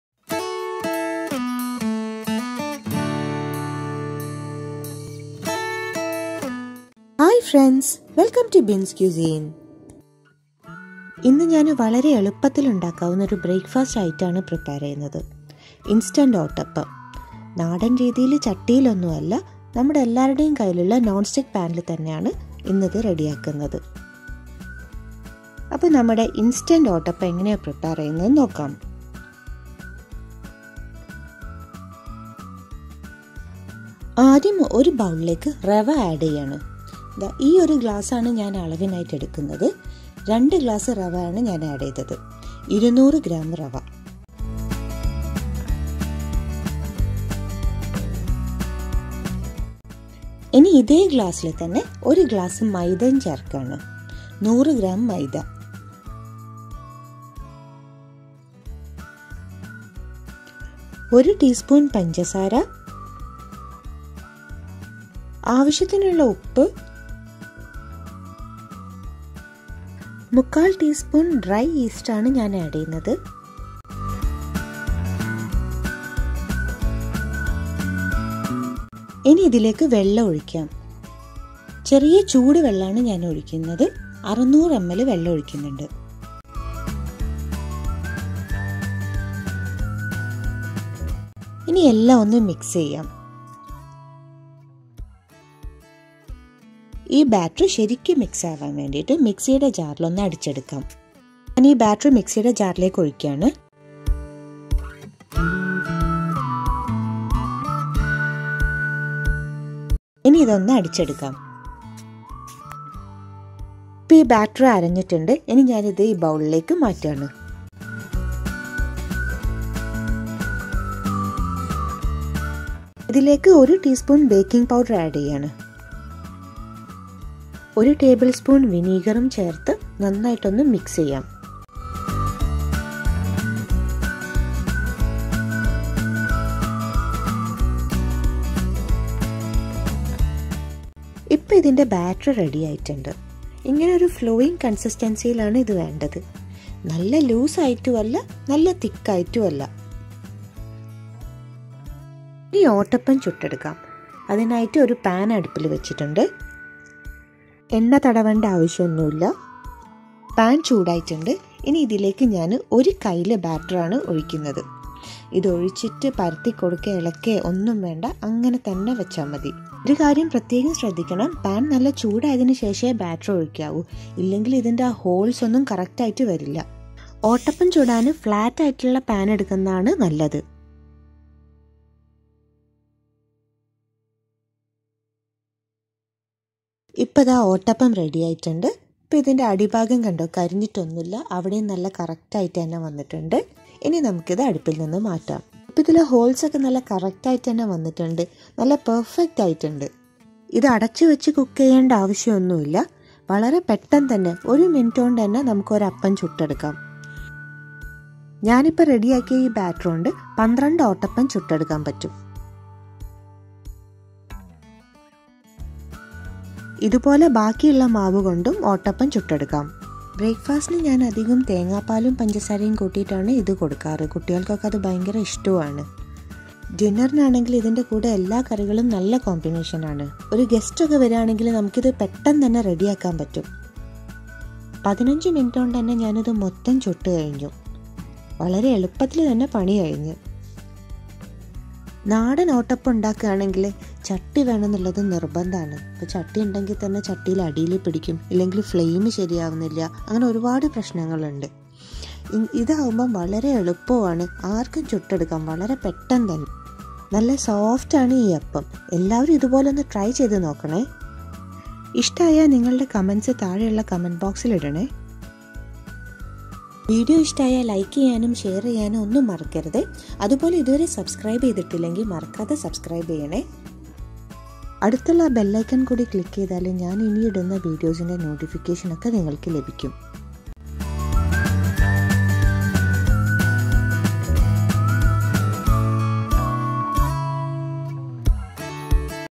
Easy Breakfast Recipes | ഇനി ഓട്ടപ്പം ശരിയായില്ലെന്ന് പറയരുത്, പാനിൽ ഇത് പോലെ ട്രൈ ചെയ്തു നോക്കൂ
Lets prepare easy instant breakfast Ottappam.

Ingredients
Rava - 200gm
Maida - 100gm
Warm water - 600ml
vinegar - 1tbsp

Follow Bin's Cuisine on your favorite social media channels